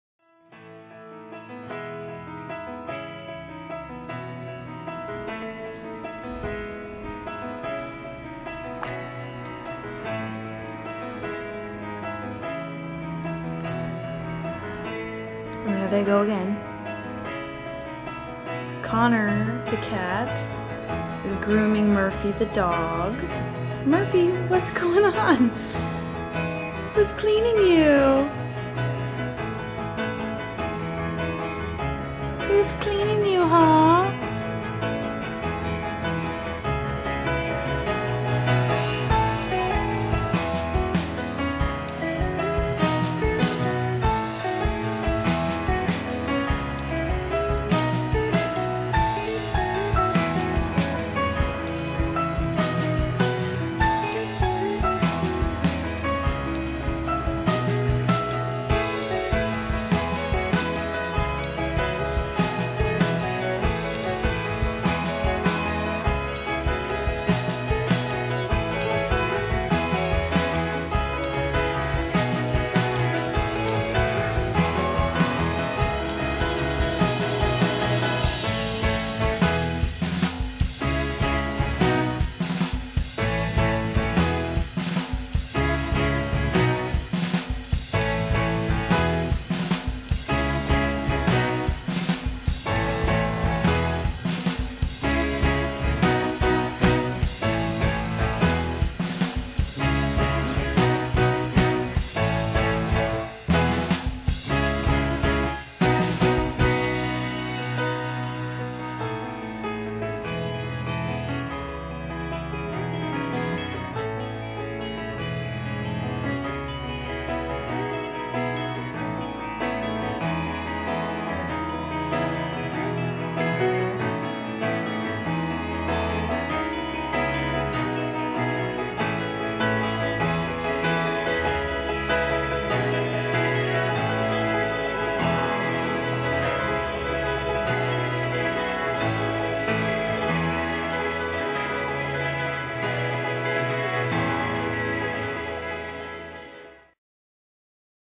Connor Grooms Murphy
this happens almost every day in my house. i finally got it on film.